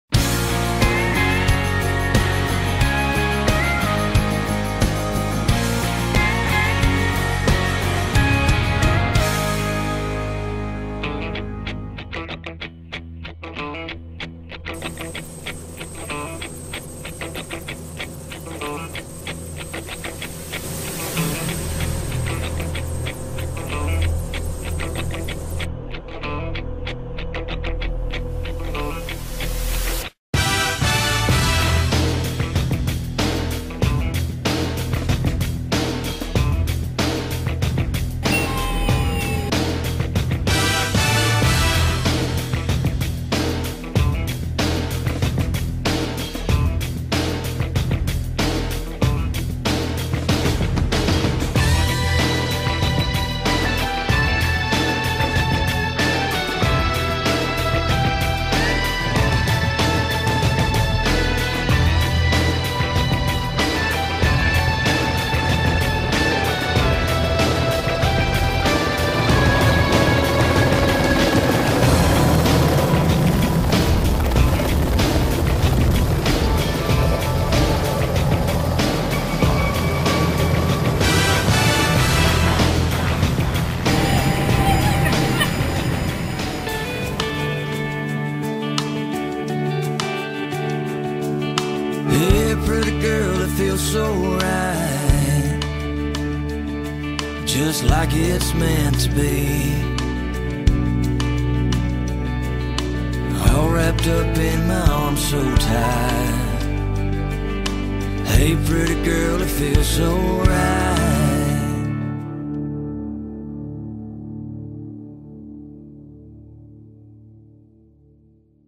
Loren Arrives at her Wedding in a Helicopter
Congratulations to Cat Country 98.1's newest bride, Loren Petisce who arrived at her wedding in Rehoboth via Helicopter ... How cool is that?  All our best to Loren and Matt